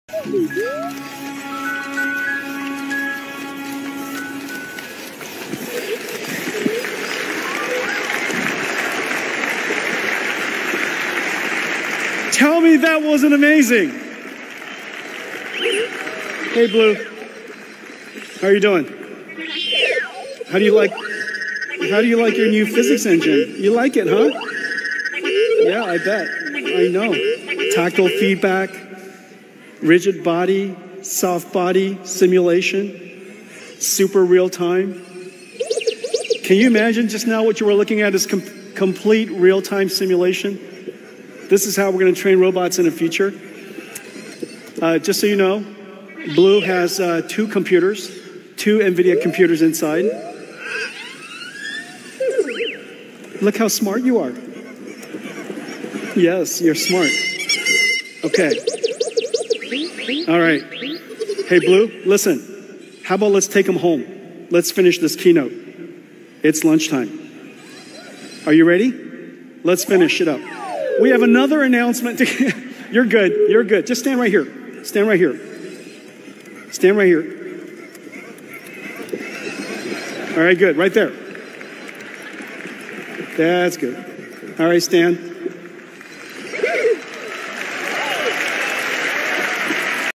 “NVIDIA CEO Jensen Huang Unveils First AI Robot | Future of Robotics Begins!”
🤖🔥 NVIDIA just changed the game — again!
Jensen Huang unveils the first AI-powered robot that learns, adapts, and evolves.
From GPUs to full-on future tech — the AI race just got real.